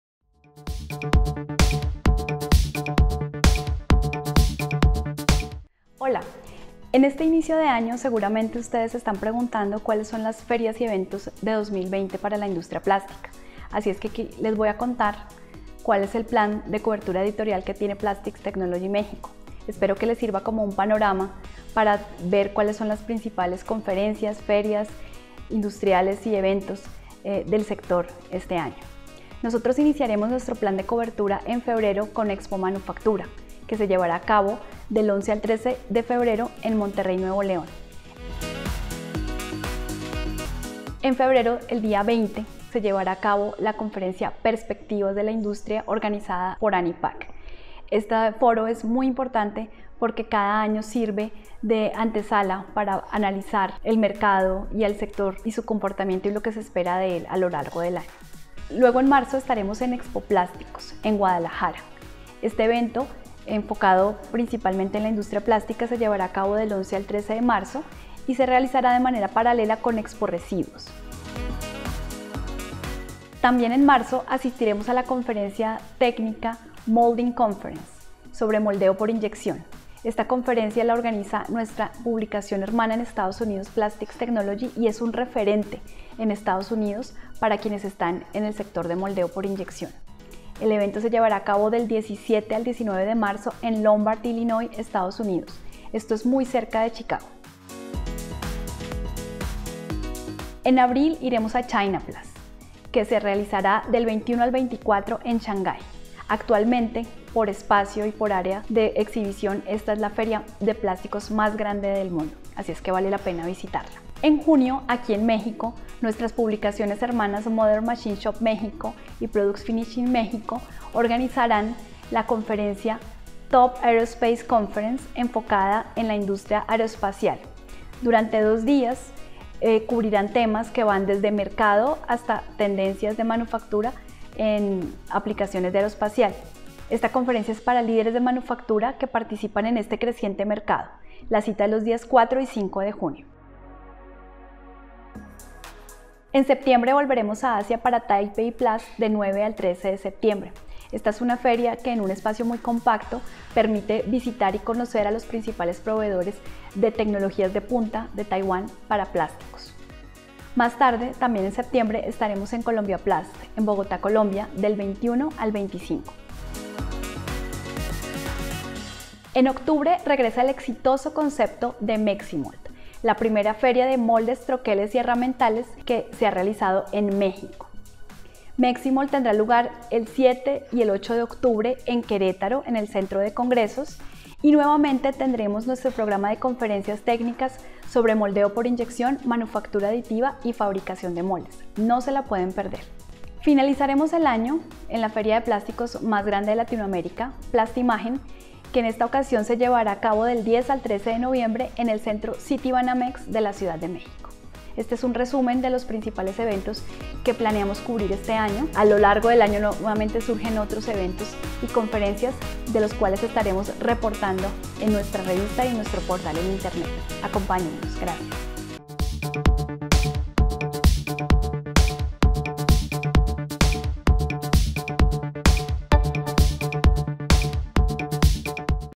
Agenda de eventos para la industria plástica en 2020
El 2020 viene con una agenda bastante nutrida de eventos de la industria plástica. Aquí les contamos cuáles son las ferias industriales, conferencias y eventos que no se pueden perder.